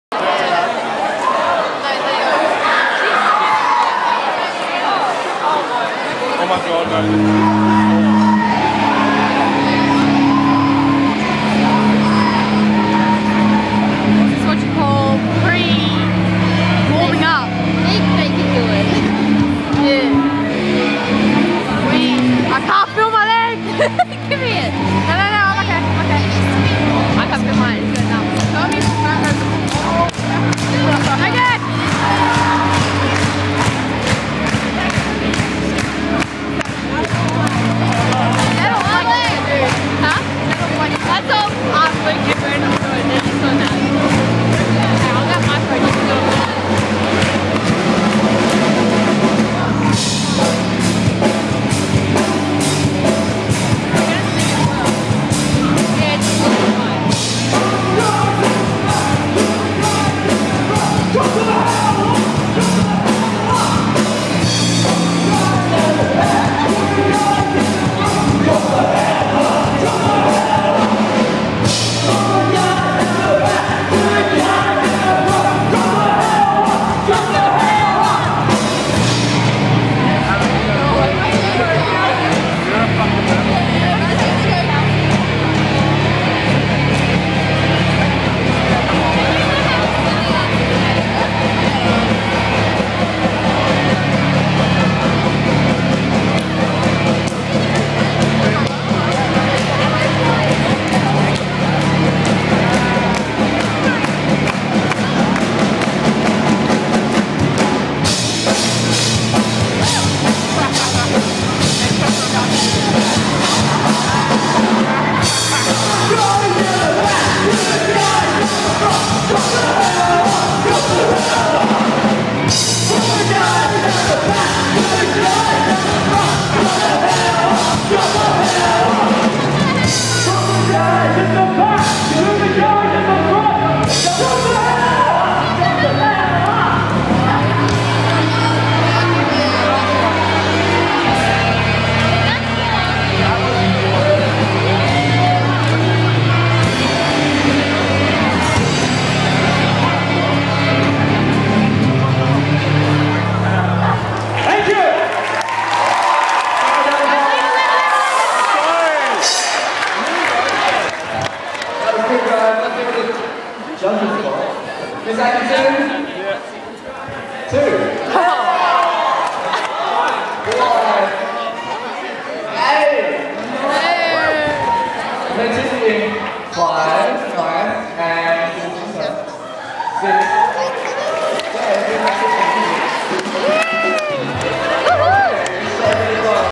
H4AH - GHS X Factor
Us playing at gorokan high's x factor